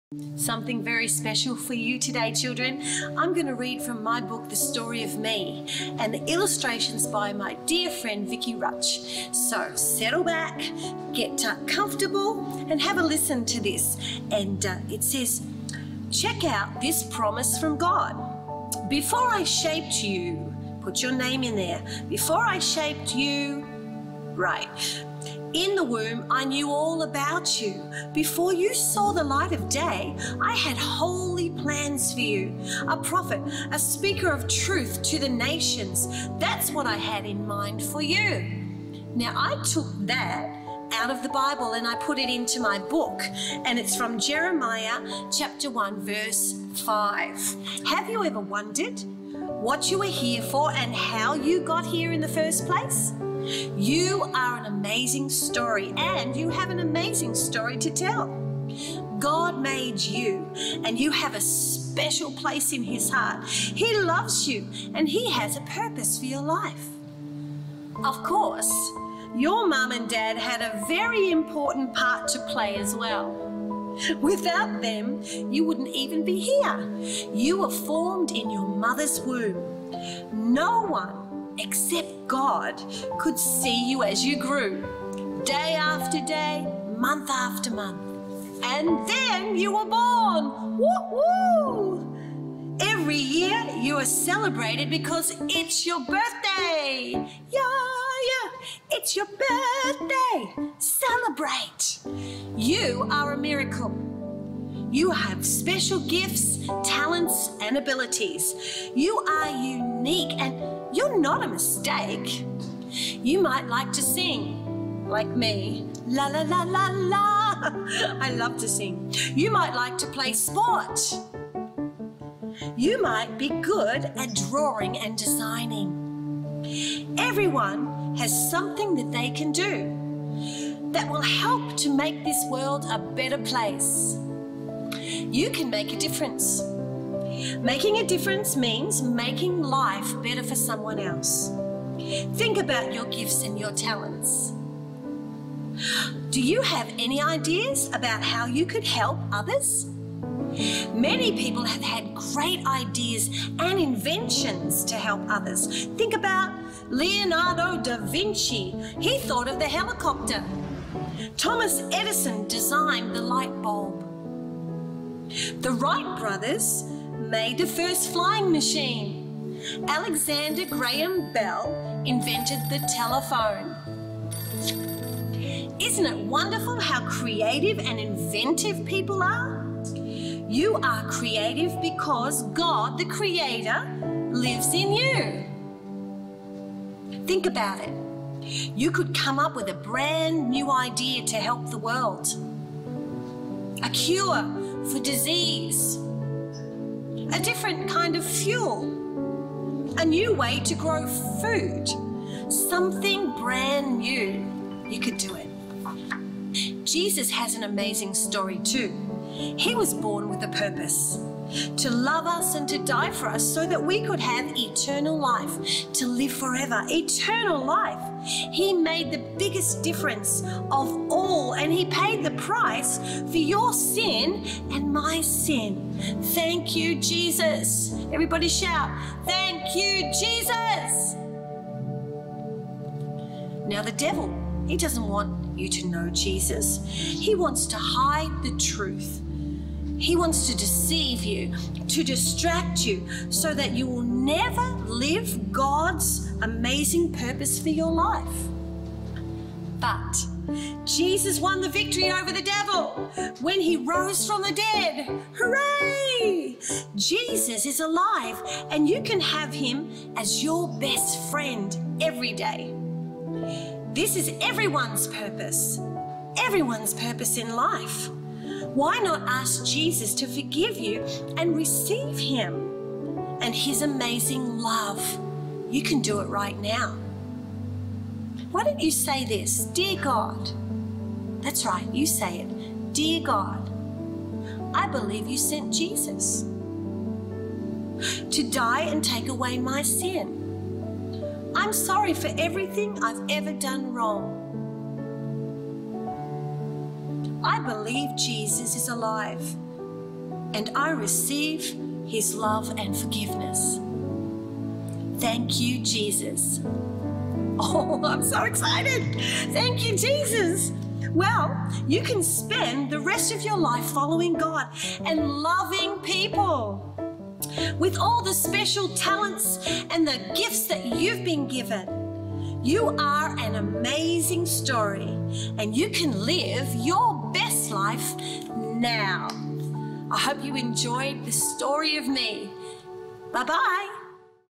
Cathie Clancy reads The Story of Me
Watch and listen with Cathie as she reads & shares her children's book 'The Story of Me'.